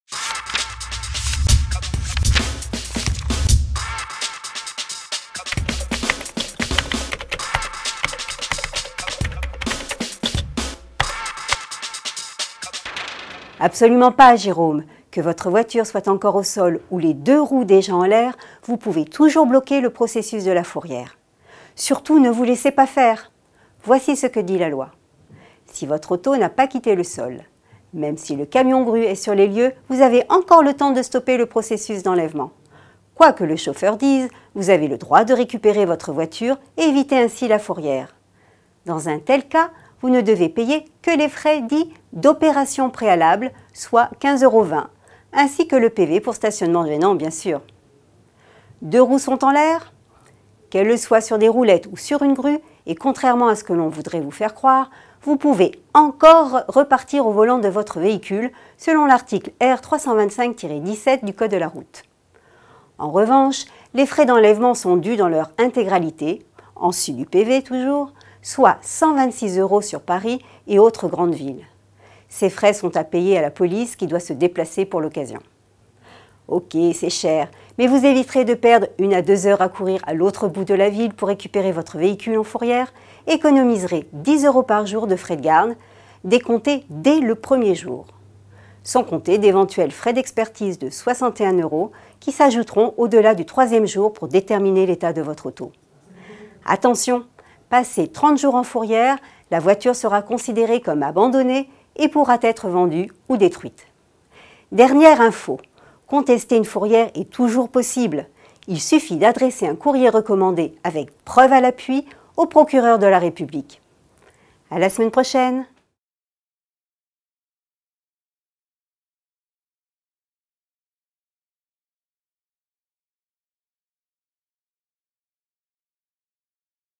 Fourrière : peut-on stopper l'enlèvement ?
Absolument pas, Jérôme. Que votre voiture soit encore au sol, ou les deux roues déjà en l’air vous pouvez toujours bloquer le processus de la fourrière. Surtout ne vous laissez pas faire ! Voici ce qui dit la loi…

1° Si votre auto n’a pas quitté le sol :

Même si le camion-grue est sur les lieux, vous avez encore le temps de stopper le processus d’enlèvement. Quoi que le chauffeur dise, vous avez le droit de récupérer votre voiture et éviter ainsi la fourrière.  Dans un tel cas, vous ne devez payer que les frais dits « d’opérations préalables »*soit 15/20€, ainsi que le PV pour stationnement gênant  bien sûr.

2° Deux roues sont en l’air :

Qu’elles le soient sur des roulettes ou sur une grue, et contrairement à ce que l’on voudrait vous faire croire, vous pouvez ENCORE repartir au volant de votre véhicule selon l’article R 325-17 du code de la route**  .  En revanche, les frais d’enlèvement sont dûs dans leur intégralité (en sus du PV), soit 126 € sur Paris et autres grandes villes***. Ces frais sont à payer à la police qui doit se déplacer pour l’occasion.

Ok c’est cher, mais vous éviterez de perdre une à deux heures à courir à l’autre bout de la ville pour récupérer votre véhicule en fourrière, et économiserez 10 €/jour de frais de garde**, décomptés dès le 1er jour ! Sans compter d’éventuels frais d’expertise de 61 €** qui s’ajouteront au-delà du troisième jour pour déterminer l’état de votre auto.

Attention, passé 30 jours en fourrière , la voiture sera considérée comme abandonnée et pourra être vendue ou détruite.

Dernière info,  contester une fourrière est toujours possible, preuves à l’appui, auprès du procureur de la République.

*Article R325-29 du code de la route

** Article R325-17 du code de la route

*** tarifs 2014. Dans les villes de moins de 400 000 habitants : frais de garde journalier à 6,18 € ; frais d’enlèvement à 116,56 €

10 jours seulement si votre voiture est évaluée à moins de 765 €.